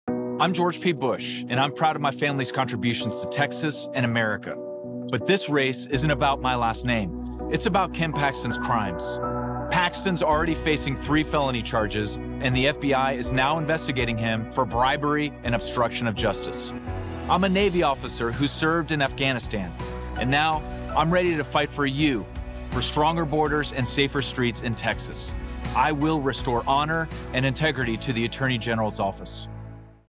Contributions
I’m George P. Bush.
I’m very proud of my family’s contributions to Texas and America.
But this race isn’t about my last name. It’s about Ken Paxton’s crimes.

Paxton is facing three felony charges…
…and the FBI is investigating him for bribery and obstruction of justice.

I’m a Navy officer who served in Afghanistan.
Now I’m ready to fight for stronger borders and safer streets in Texas.

I will restore honor and integrity to the Attorney General’s office.